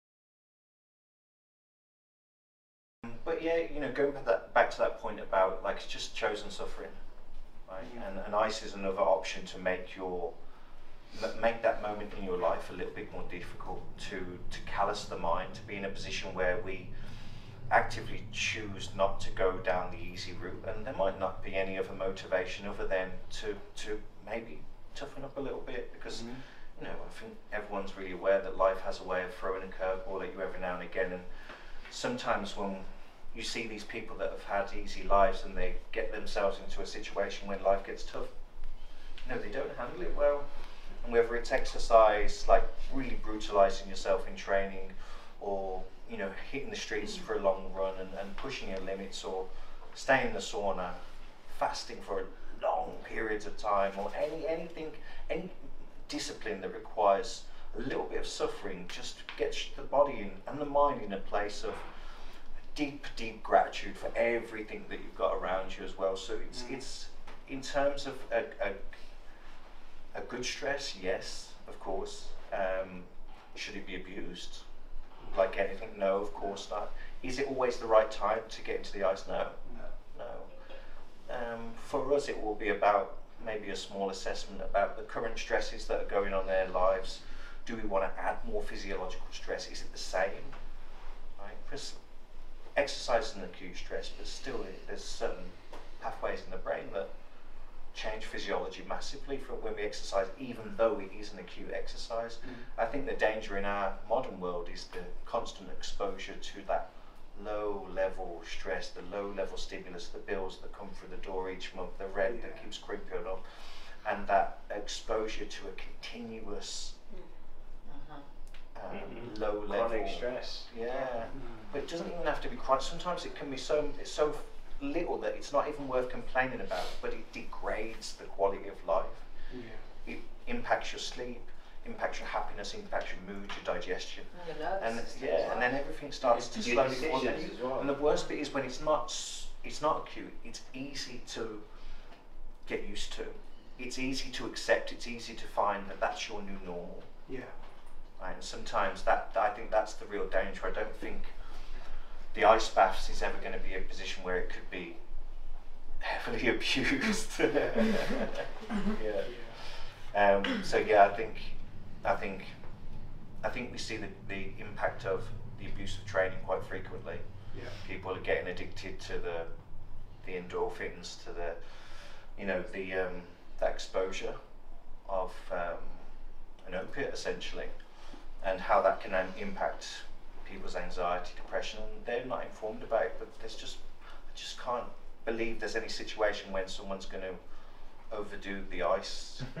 What do you believe about the cold in terms of resilience building? | Lawrence Fountain
We ask Lawrence Fountain: The cold as a hormetic stressor, like exercise, what do you believe about the cold in terms of resilience building?
Our gym expert compares the two disciplines as forms of stress and the relief they bring.

Laurence is the owner of Salus London. An exclusive gym that provides elite training, body conditioning, and lifestyle coaching. He discovered the power of cold water immersion through a friend and could say the rest is history, but actually, it was more like the beginning of the future.